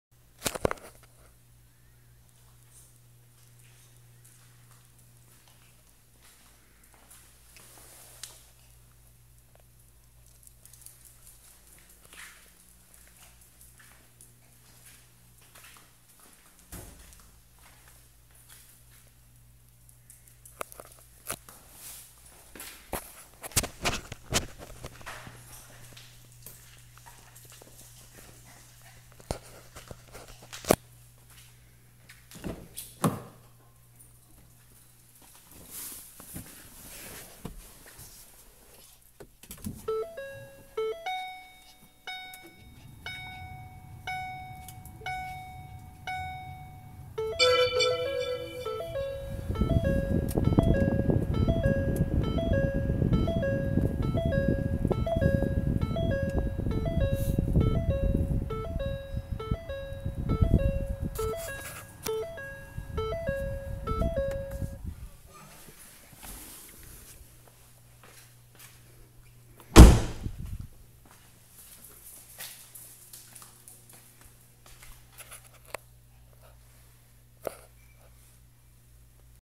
New 2013 Ford Focus Titanium
Just a quick walk around of my new Focus Titanium